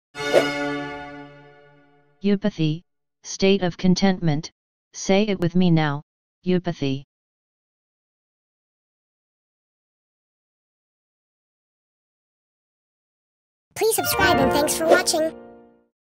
eupathy - Smart & Obscure English Words Defined 🗣🔊
This video is about "eupathy". Here's a video list of some of the most obscure english words with definitions that you might use as a curiosity in your conversations.